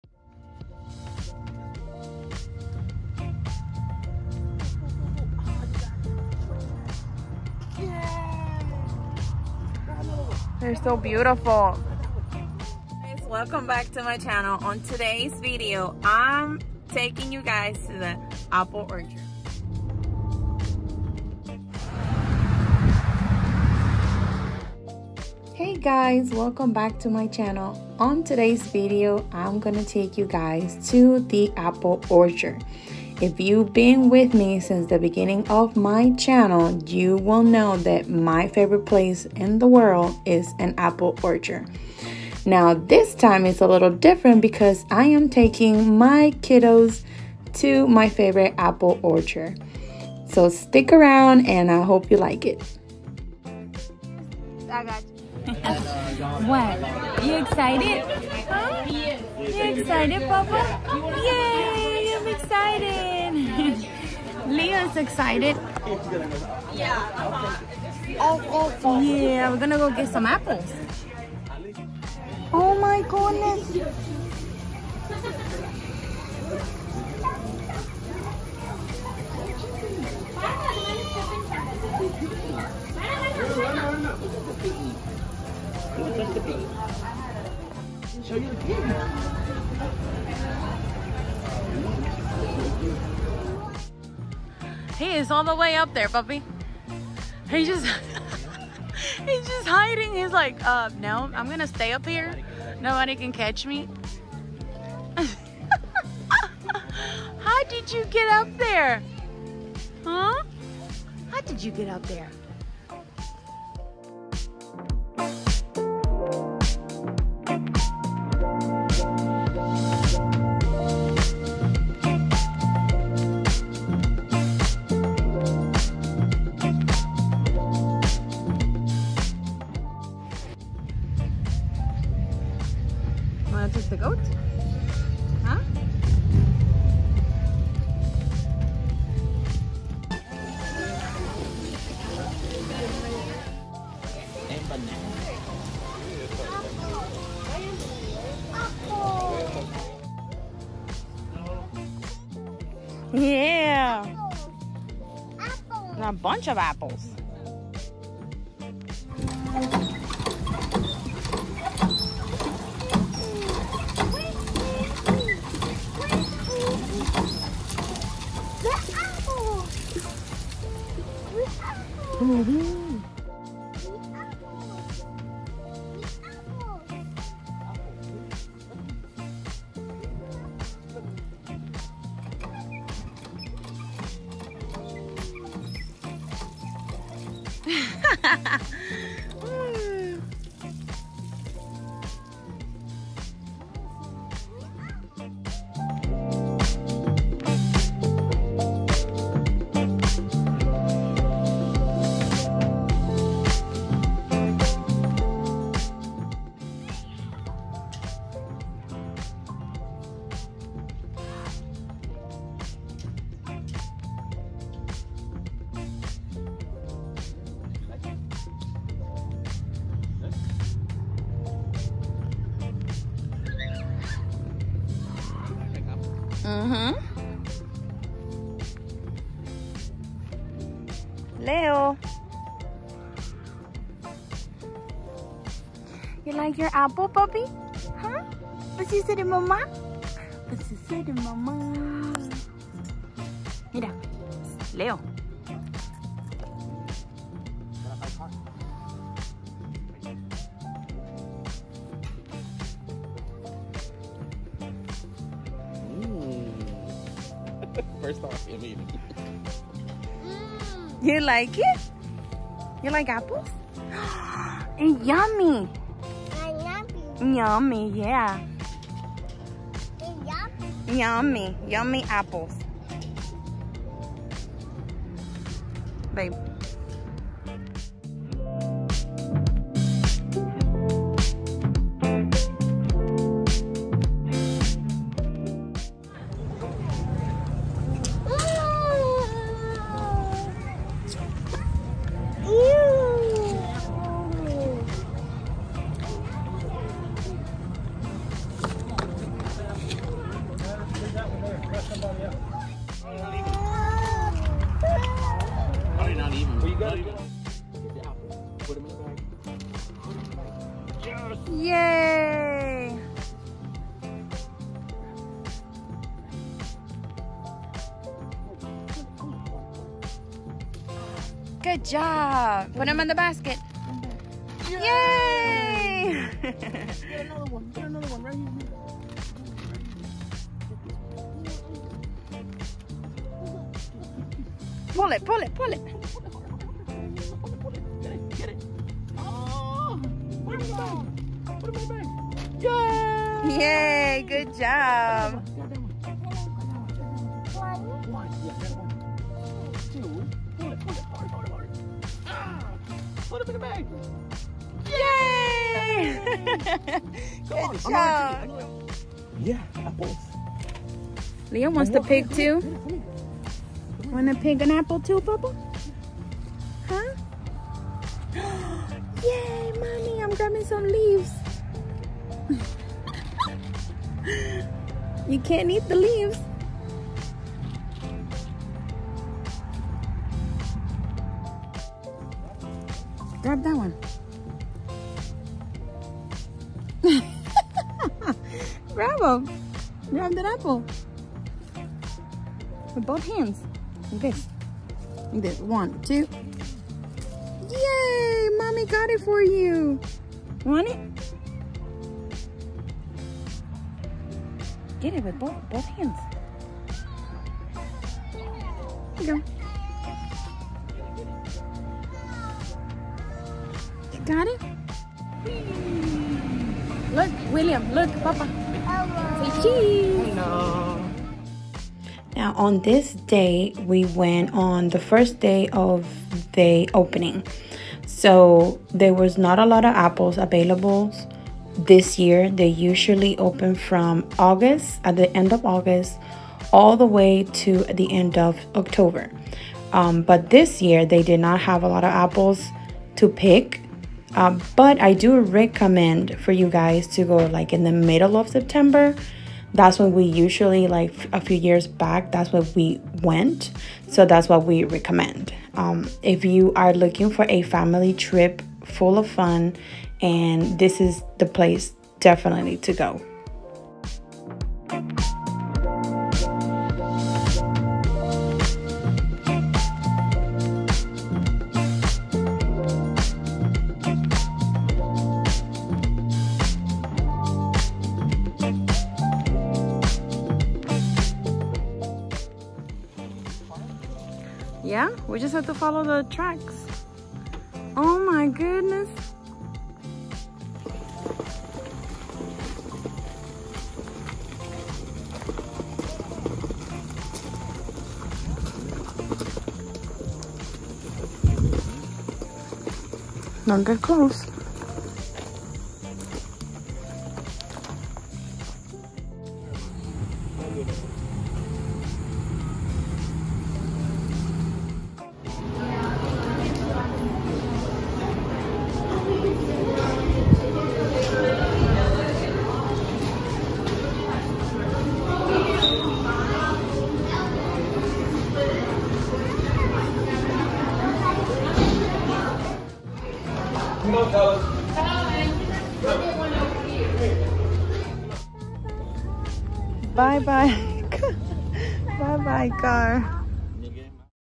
Apple Picking at B.J. Reece Apple Orchard 🍎
On today’s video, I’ll be taking you guys with us and show you a very special place to me, specially now that I get to take the kiddos. Hopefully you can also visit them someday.
If you are looking for a great fun get a way with your love ones, this is the place to go.

If you guys are in the area visiting the beautiful mountains of Georgia make sure you guys give them a try. You will not be disappointed.

Xoxo